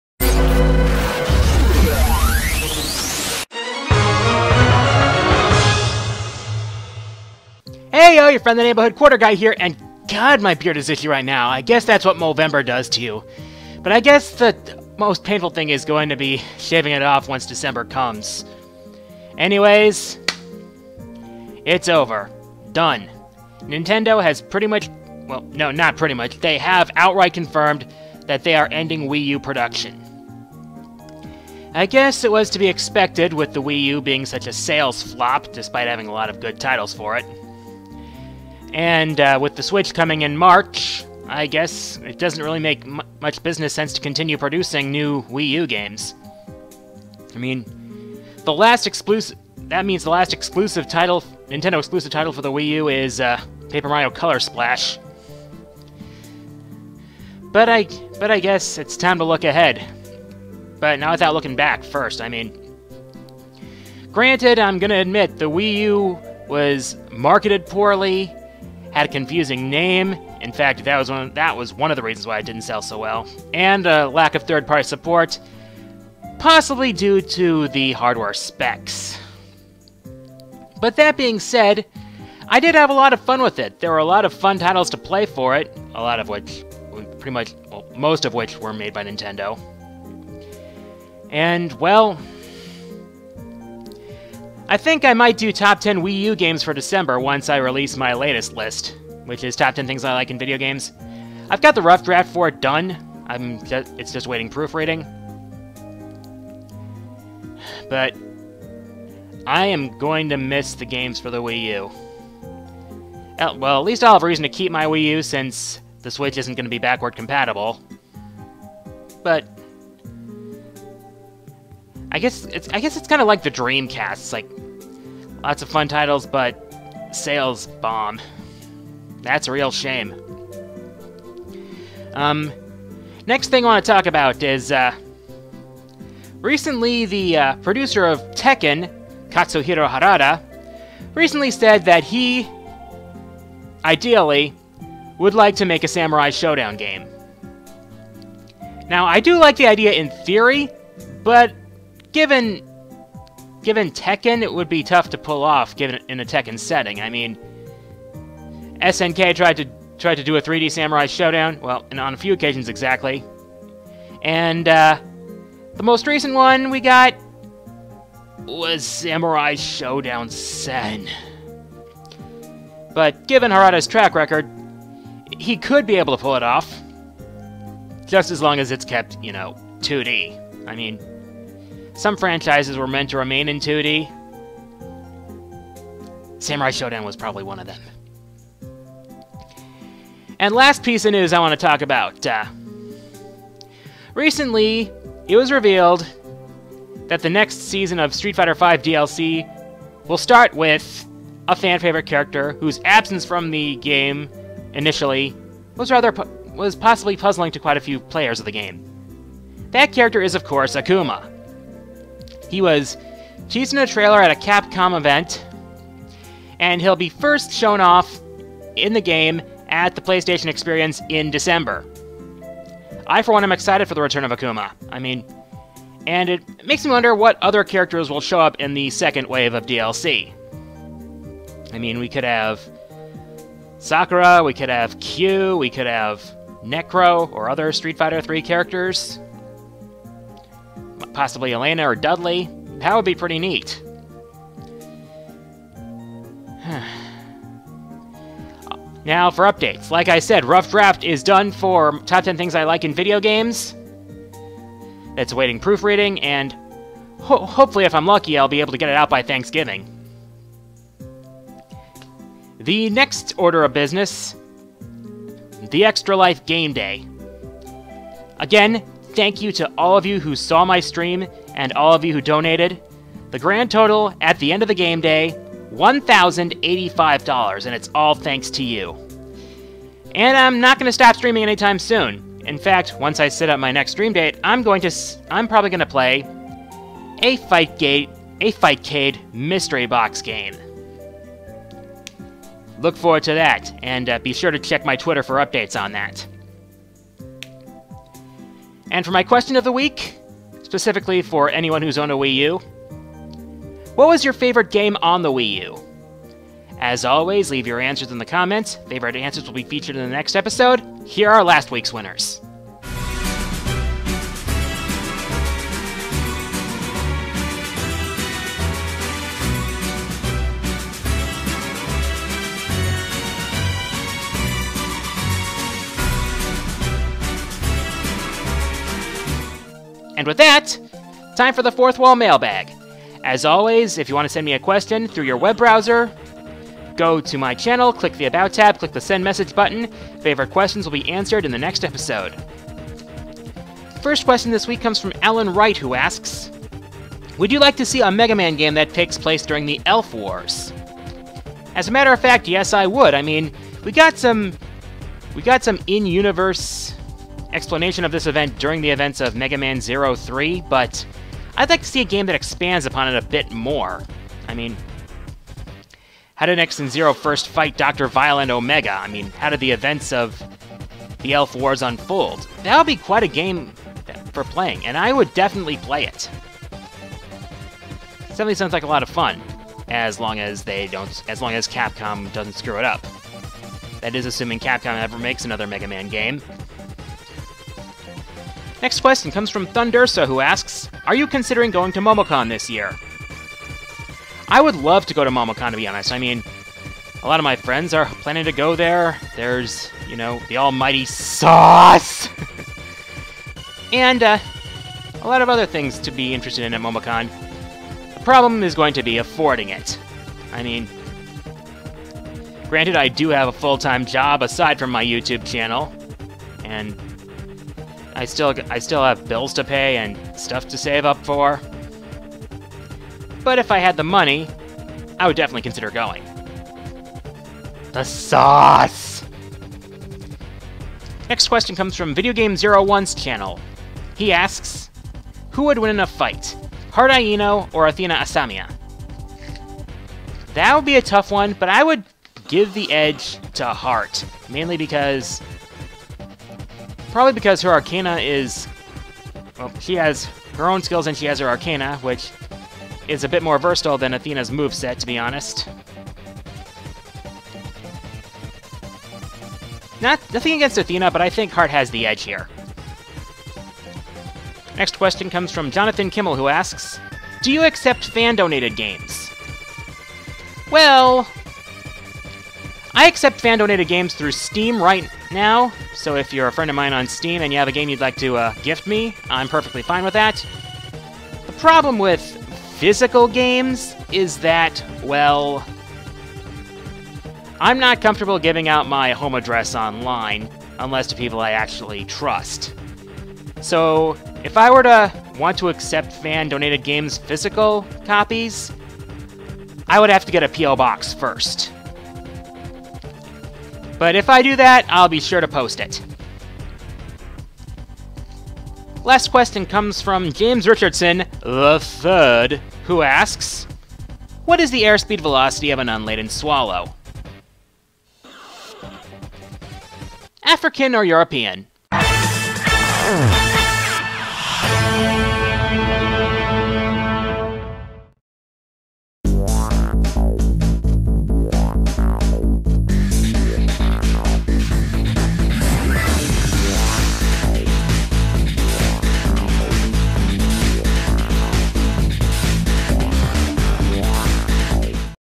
My 25 Cents Worth 11/13/2016 - RIP WiiU
It's been real, but we all knew this was coming.  The WiiU was a sales flop, despite some awesome games, and with the Switch on the way, there's no point in continuing to make more WiiU systems.